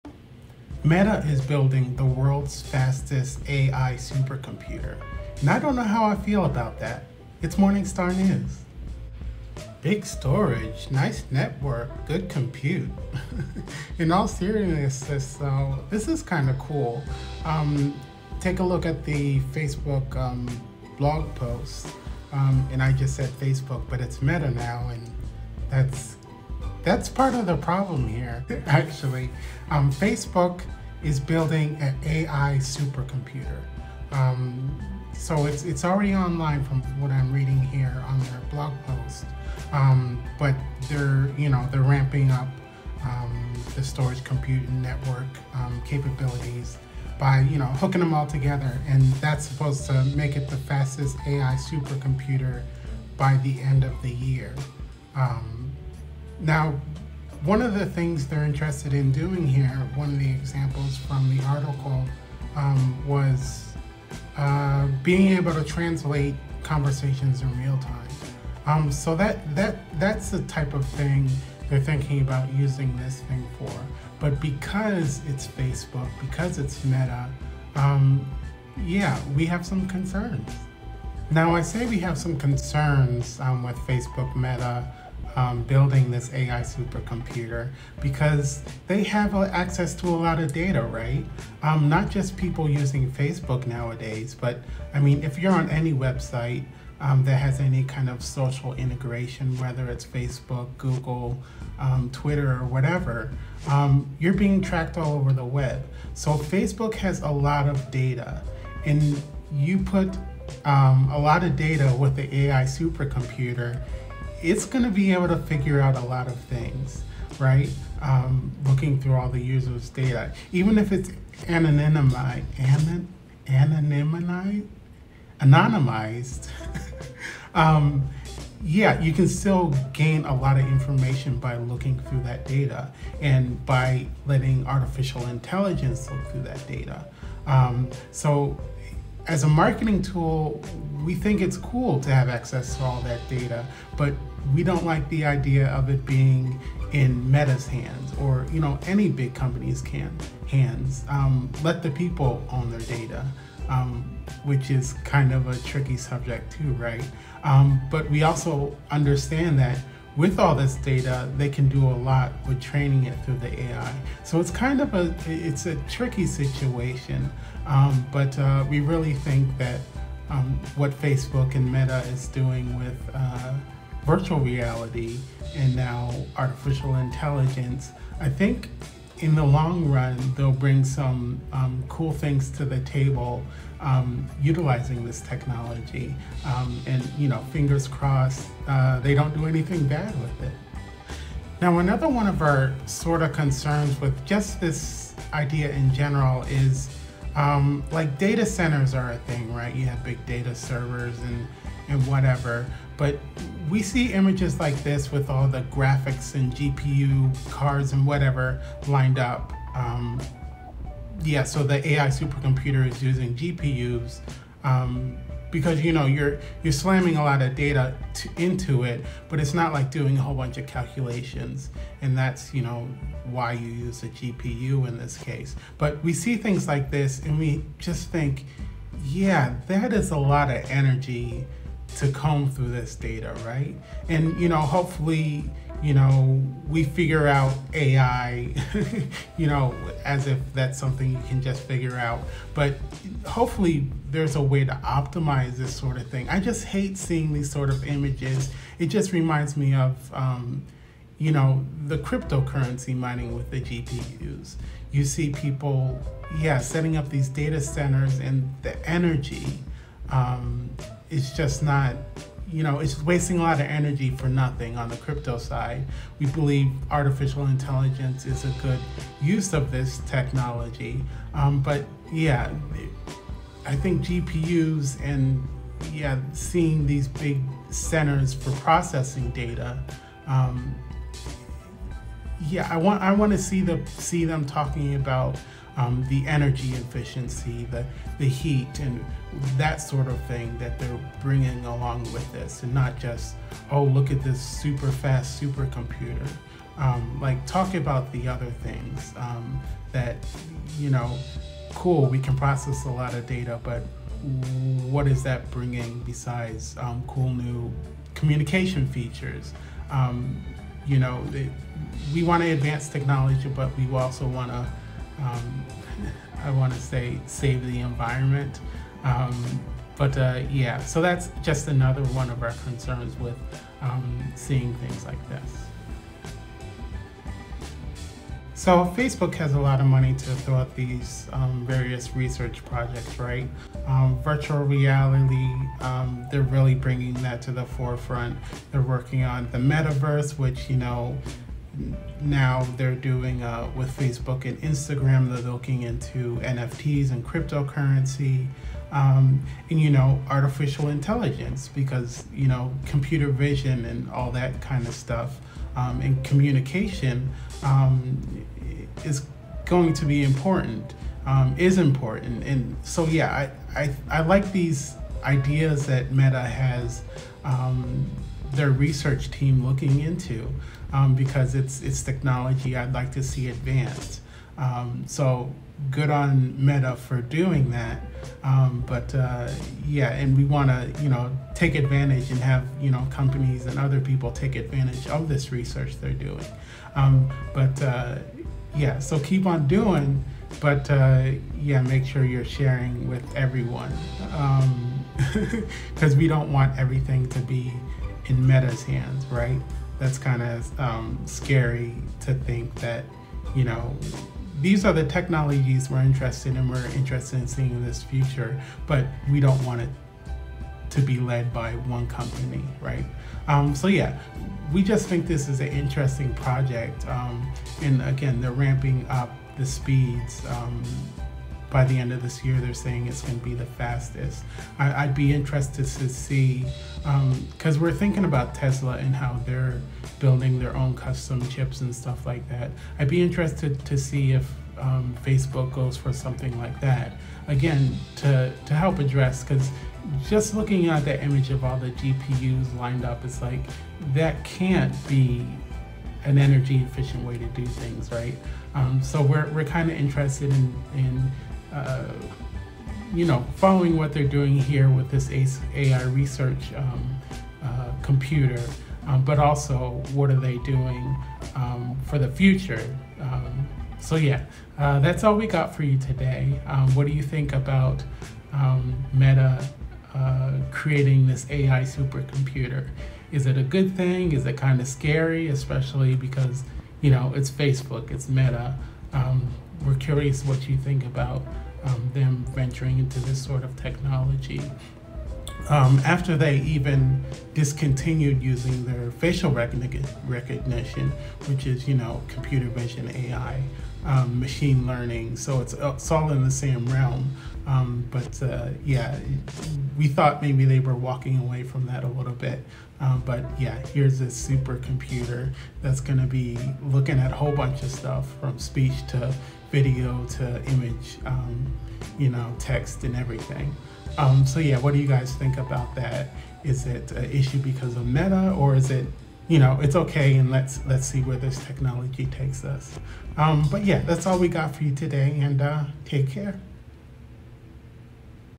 Morning Star 🌟 News - Meta Is Building The World's Fastest AI Supercomputer (2022.01.25)
Welcome to Morning Star News

00:00 - Intro
00:09 - Meta's AI Research SuperCluster
Introducing the AI Research SuperCluster — Meta’s cutting-edge AI supercomputer for AI research
Meta has built an AI supercomputer it says will be world’s fastest by end of 2022

01:21 - Doing A Concern About Data
03:11 - Doing A Concern About Energy
05:28 - Putting Your Money Where Your Meta Mouth Is
07:46 - What Do You Think?
08:43 - Outro

Take care...